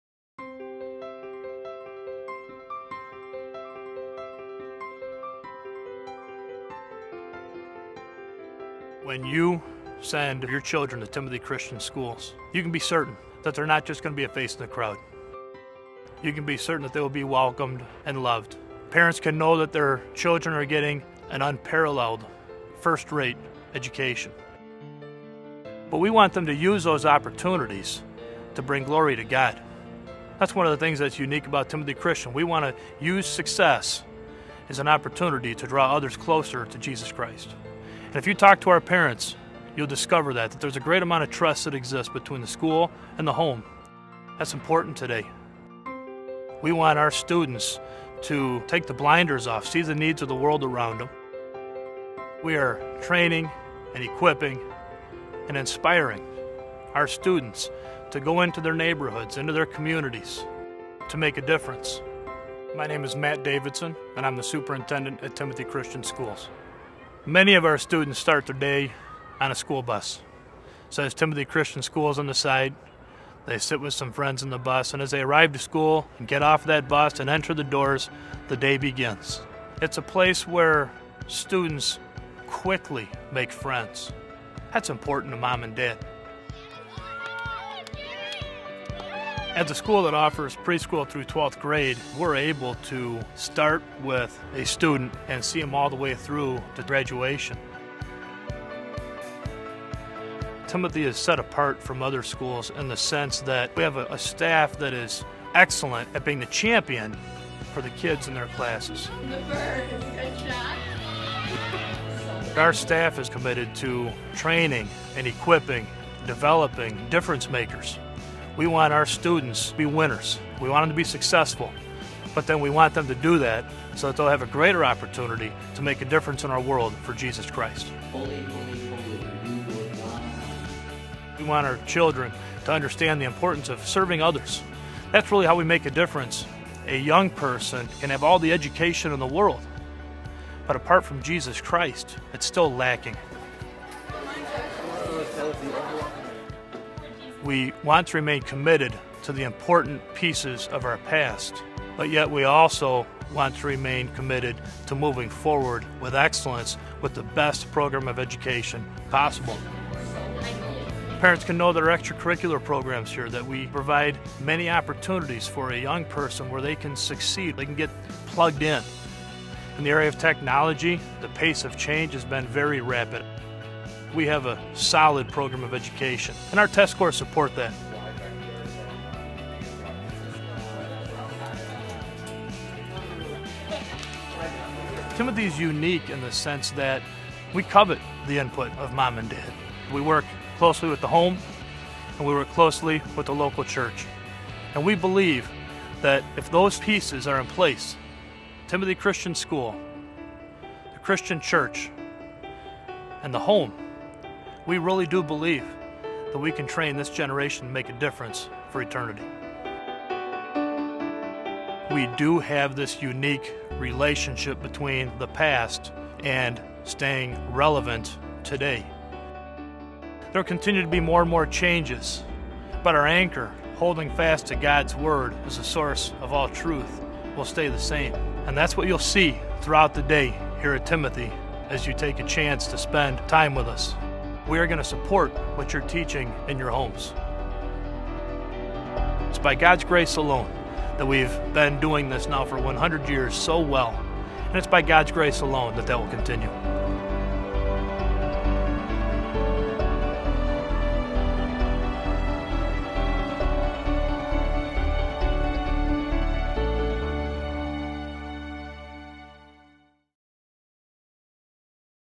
Timothy Christian Schools Video
Superintendent Matt Davidson helps new families explore a Timothy Christian education.

Founded in 1911, Timothy Christian Schools is a fully accredited, parent-run, P-12 private Christian school system in Elmhurst, IL with academic excellence and a reformed Biblical worldview at the heart of its mission.

Timothy Christian Schools: 100 Years.  1 Mission.